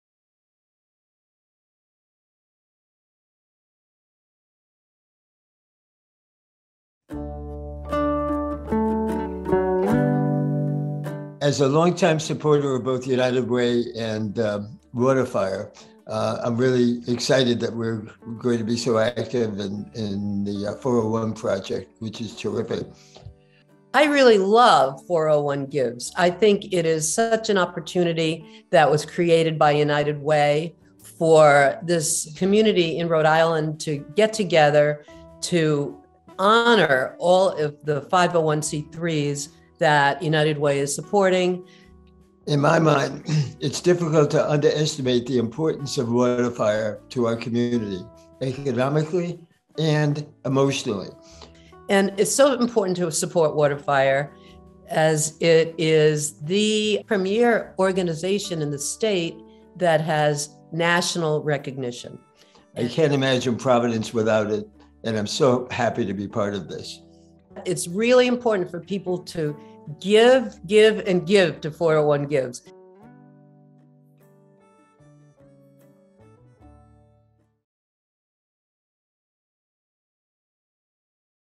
401Gives 22 Waterfire
Elaine and Barry Fain share insight about Waterfire.